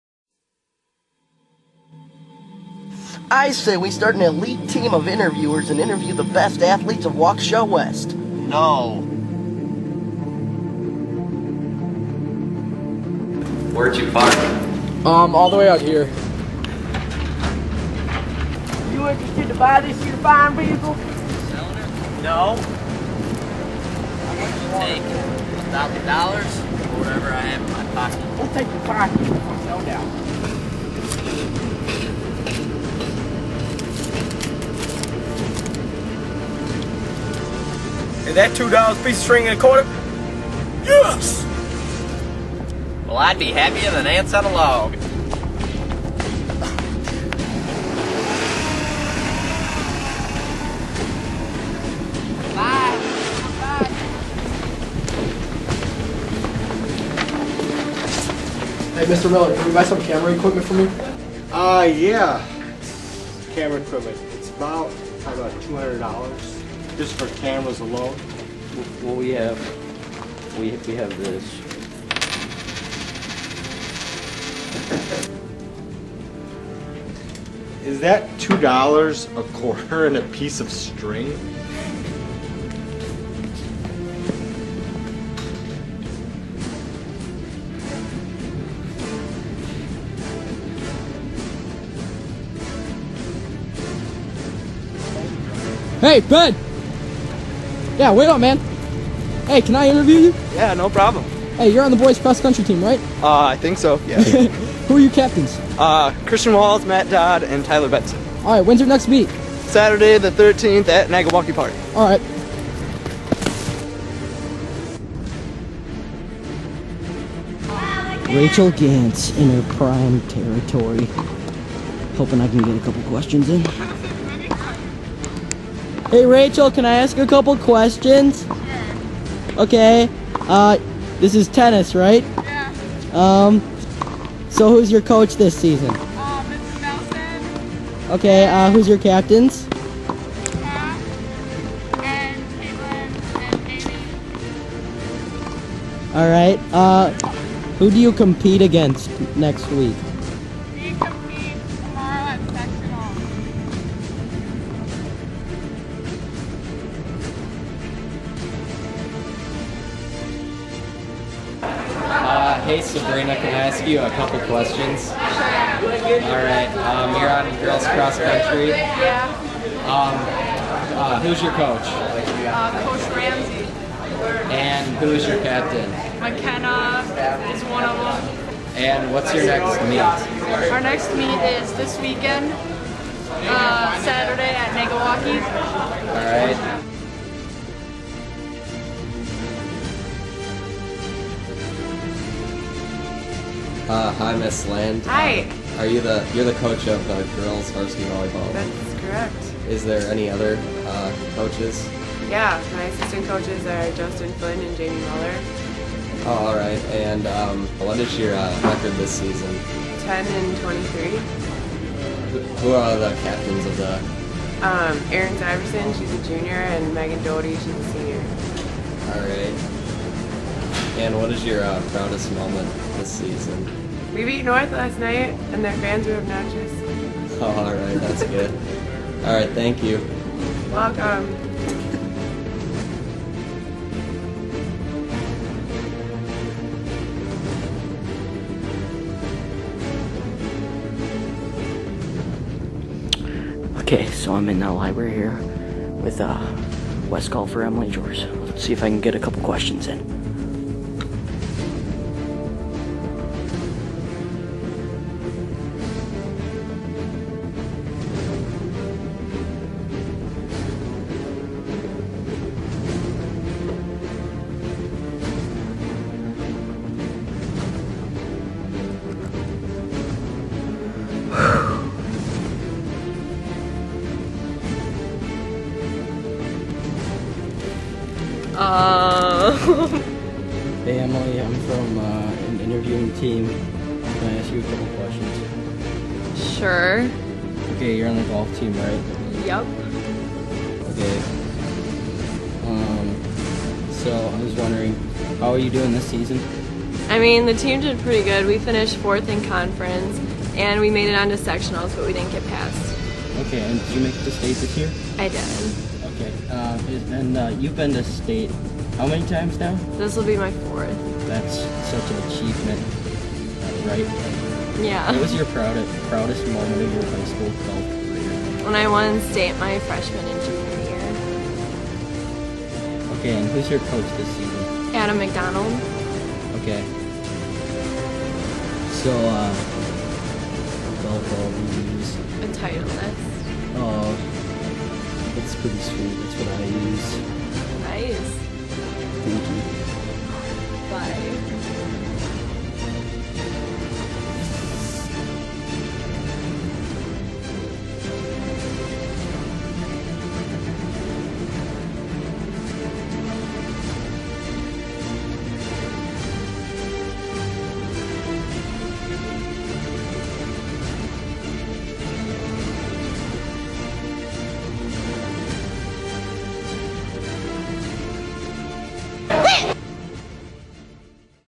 Fall Sports Part 2
This video highlights the 2012 fall sports teams at Waukesha West High School